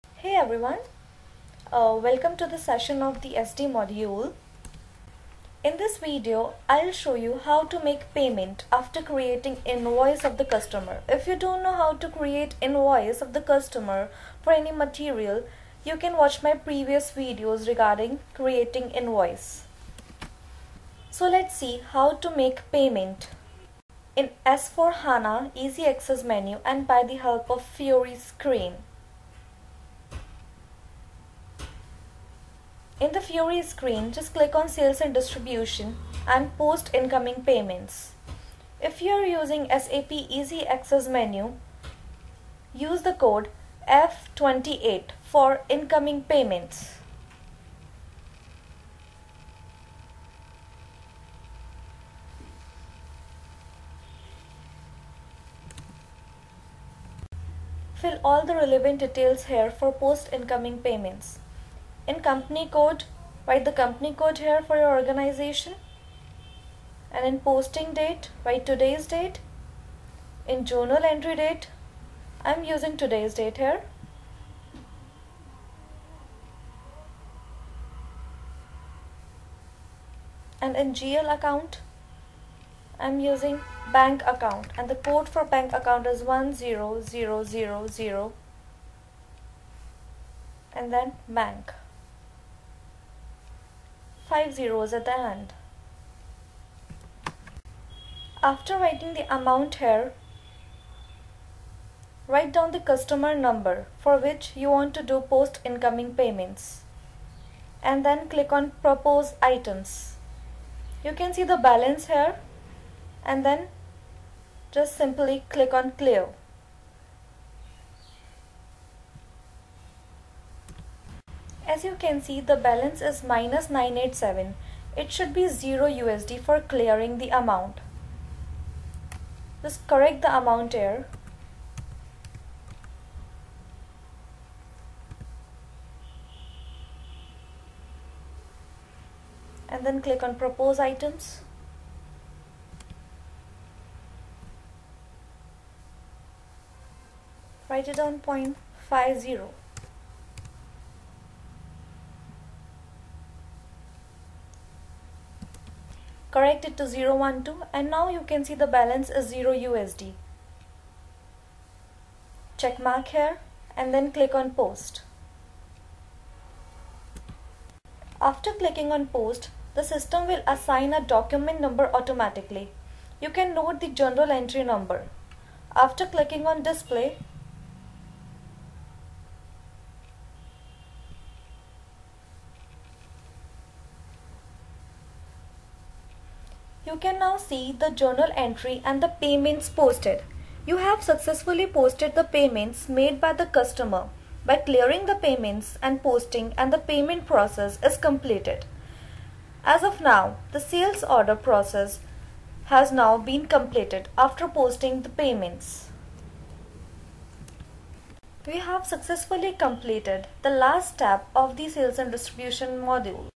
How to perform post incoming payments in S/4HANA - Post incoming payments or clearing payments FIORI
This video is all about explaining the post incoming payment process after clearing the invoice in the latest S/4HANA screen.
F-28 is the TCode to perform the task post incoming payments through SAP Easy Access Menu.